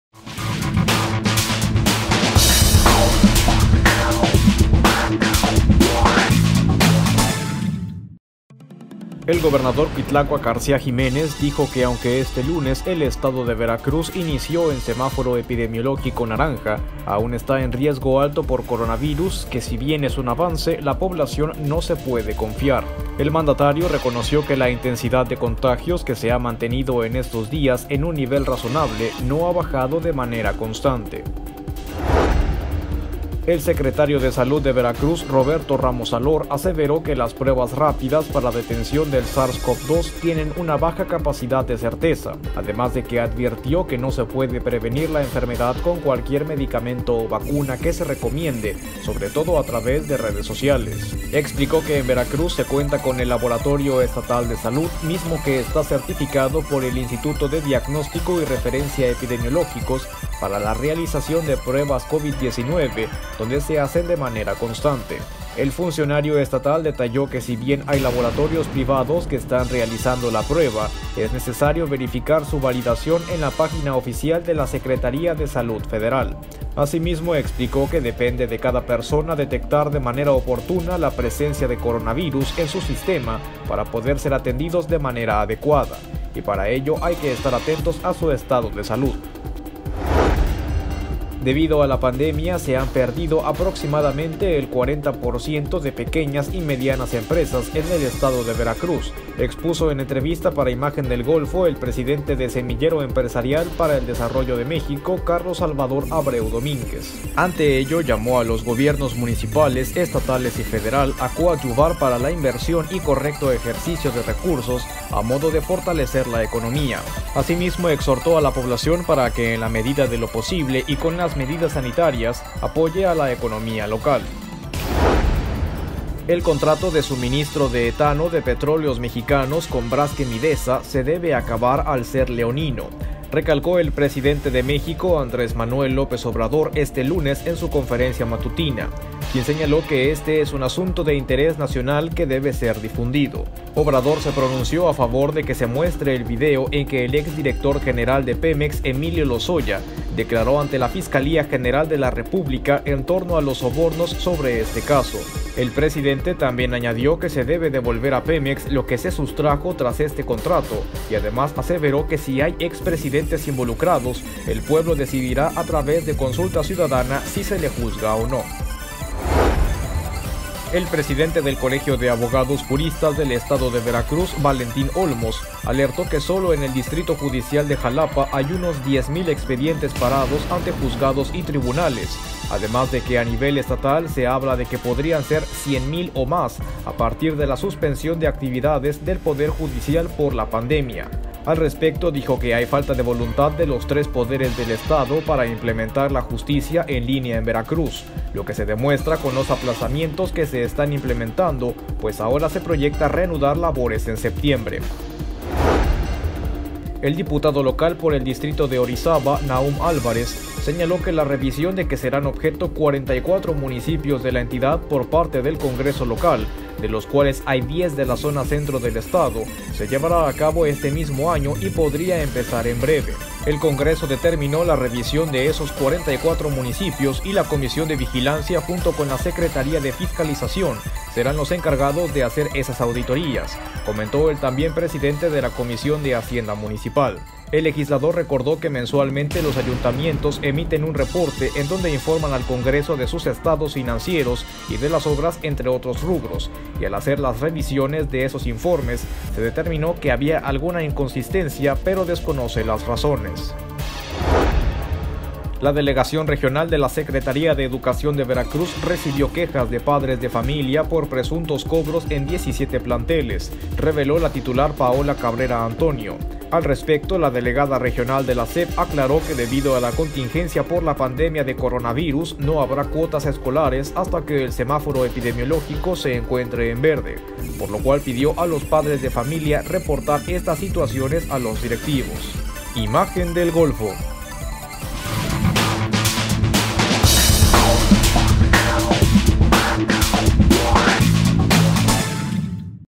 RESUMEN: AGOSTO 17
lunes, 17 de agosto

🚥 Aún hay riesgo alto por covid
🦠 Pruebas rápidas no son certeras: Salud
🏬 Cierran 40% de Pymes en Veracruz
🏭 Debe acabar contrato con Braskem: AMLO
🔎 Congreso Local revisará 44 municipios

🎙️: Fernando Plata